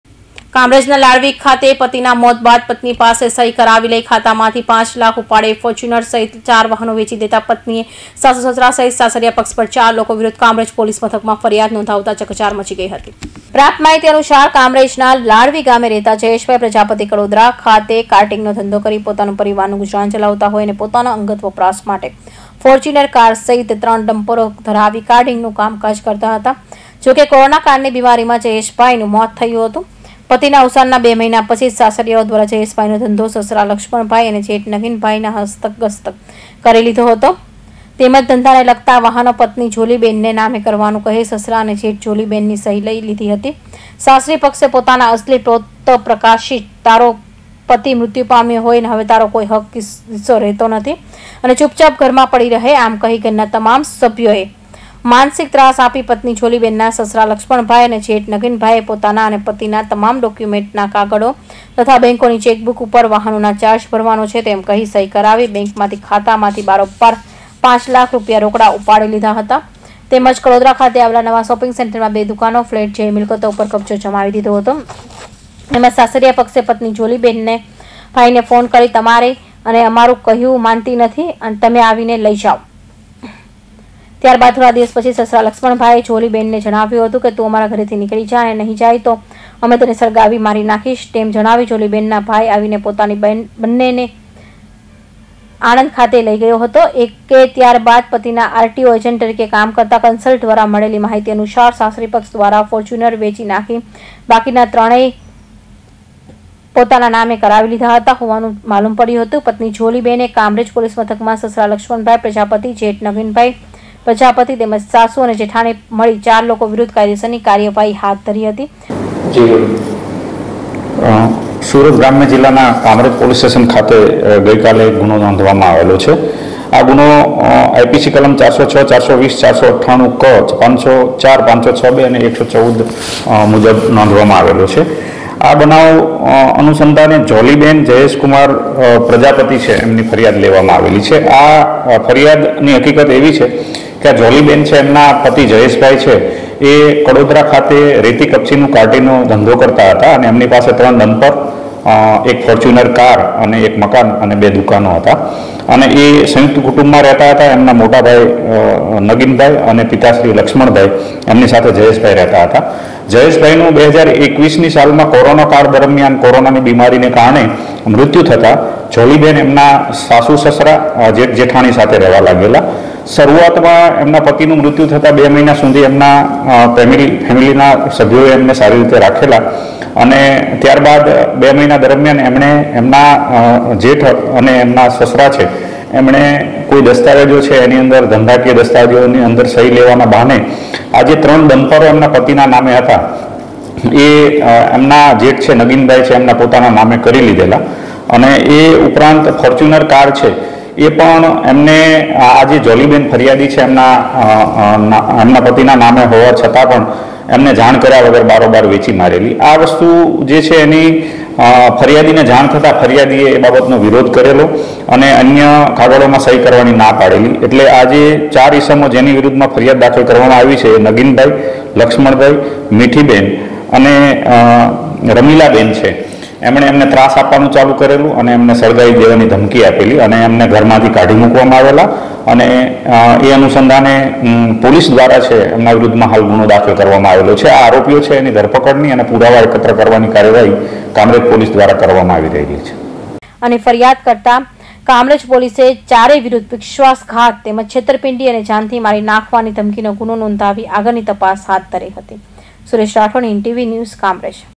સુરતના કામરેજ વિસ્તારની ઘટના
પતિના મોત બાદ વહુ પાસે સહી કરાવતા સાસરીયાપક્ષ વિરુદ્ધ ફરિયાદ
ચાર લોકો વિરુધ કામરેજ પોલીસ મથકે ગુનો દાખલ થતા ચકચાર